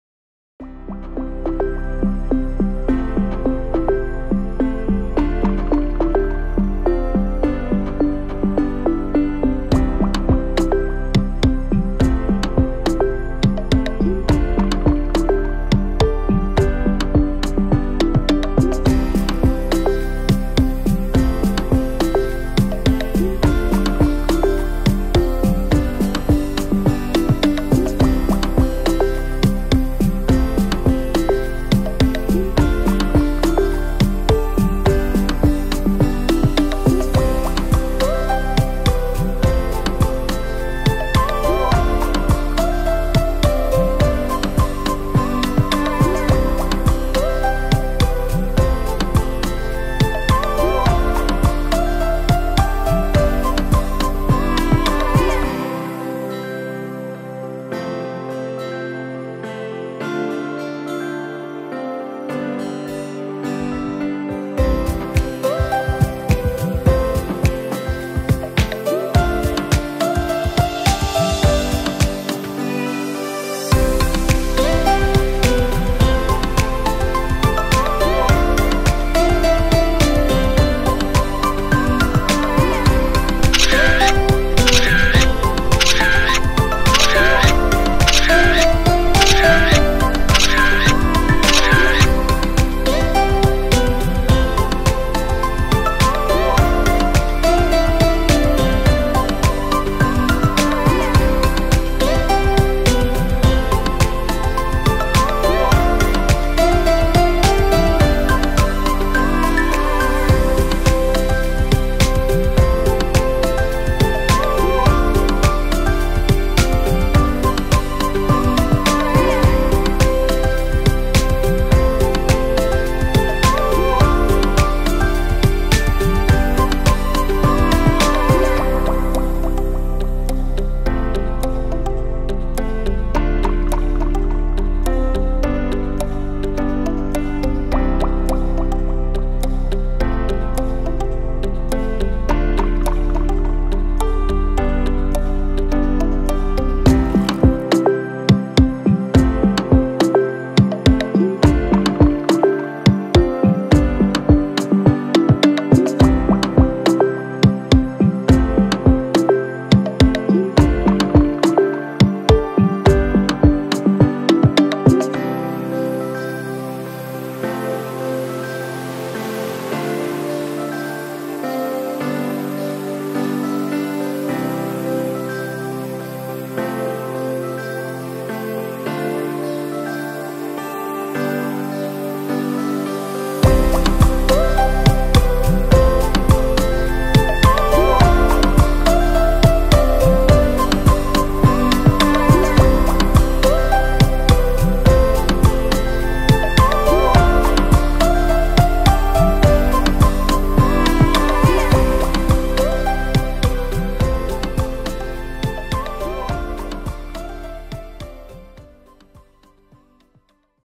Forest Library in Taiwan? Pingtung County Library Tour with Taiwan News [Taiwan News Travel Series]
Pingtung Country Library turns into a forest library after a 2-year renovation. Follow Taiwan News today for a quick tour inside the library!

👉 Photo credits: Photos of the library's 5th-floor auditorium are provided by Pingtung County Library